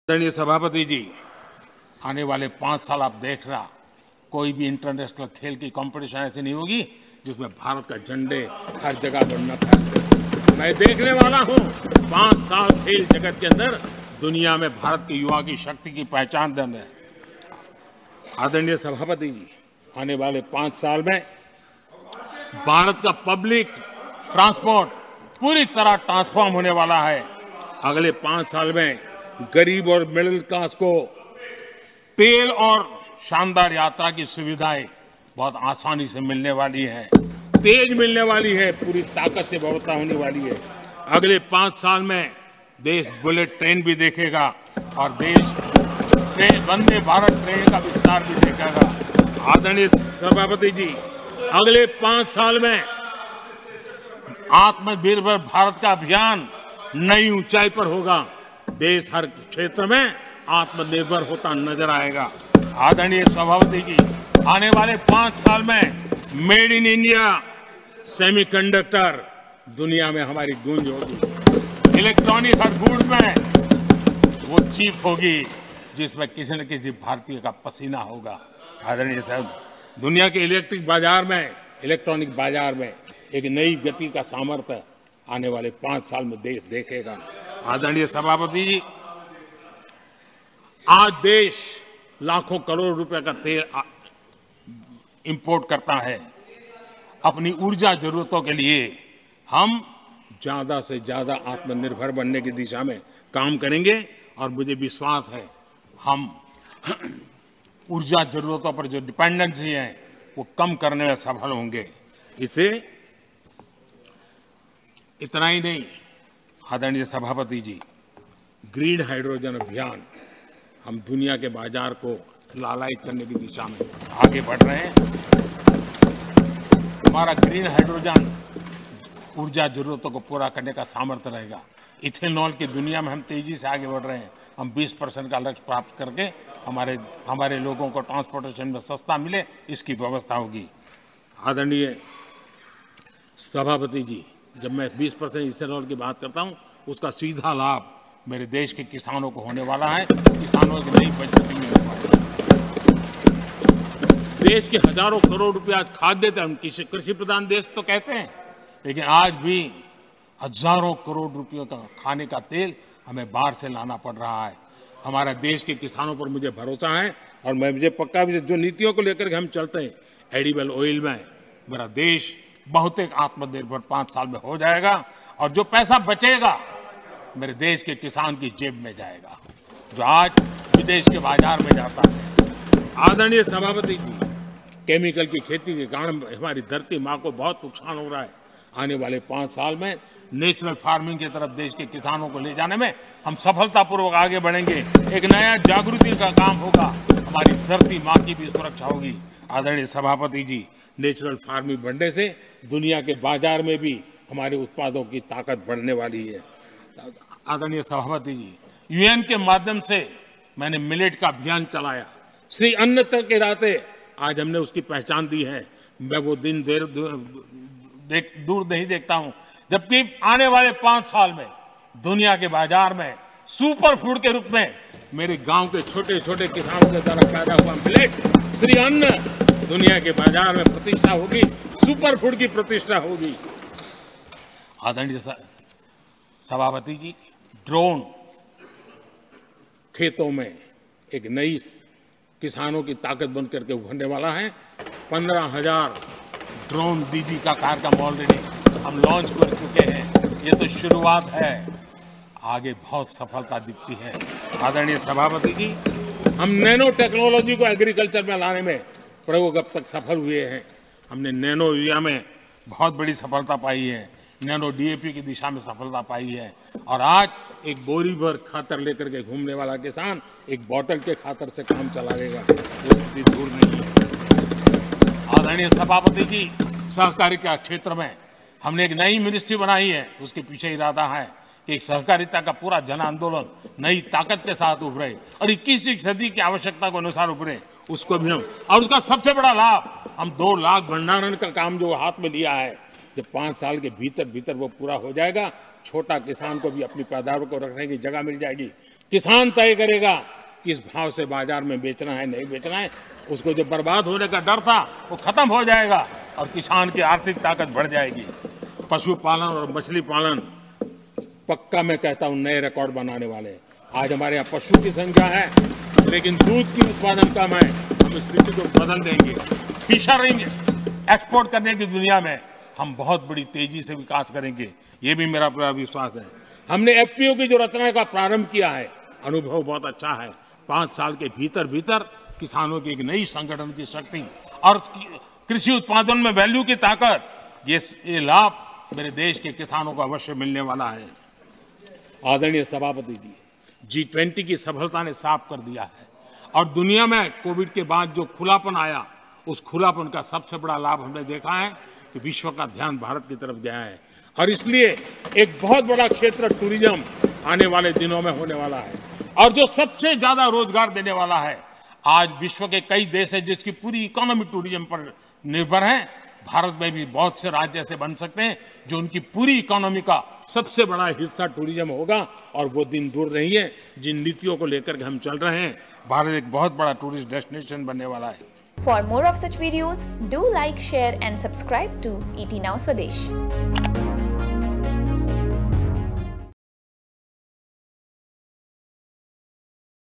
PM Modi Speech In Rajya Sabha : पीएम मोदी ने बताया अगले 5 साल का क्या है Plan? Hindi News | Swadesh
पीएम मोदी ने कहा हमारी सरकार का तीसरा टर्म दूर नहीं है। पीएम मोदी ने कहा कि कुछ लोग इसे मोदी 3.0 कहते हैं। मोदी 3.0 विकसित भारत की नींव को मजबूत करने के लिए पूरी शक्ति लगा देगा। अगले 5 साल में देश में इलाज बहुत सस्ता हो जाएगा। आने वाले 5 साल में गरीब को पीएम आवास देने का ख्याल रखा जाएगा। अगले 5 साल सोलर पावर से बिजली बिल जीरो होगा। अगरे 5 साल देश में पाइप से गैस के कनेक्शन पूरे देश में नेटवर्क बनाने का पूरा प्रयास किया जाएगा। युवा शक्ति बनेगा देश।